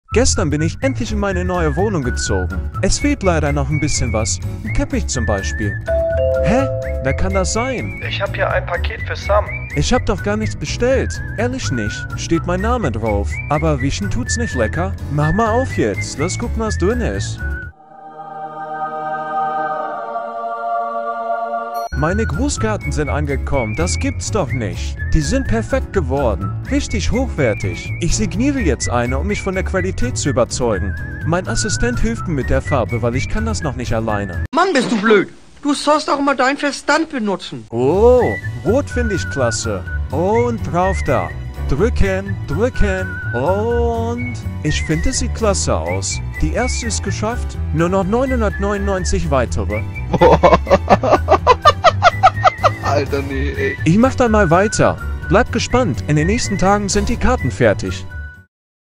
Paket für meinen Hund 🐶
Sam ist gerade in seiner neuen Wohnung eingezogen, als es plötzlich an der Tür klingelt.

Ein Paket für Sam!

Aufgeregt nimmt Sam das Paket in Empfang. Was ist denn da?
Die Autogrammkarten sind endlich angekommen!

Jetzt kann der Spaß erst richtig losgehen.

Bleib gespannt wie es weitergeht!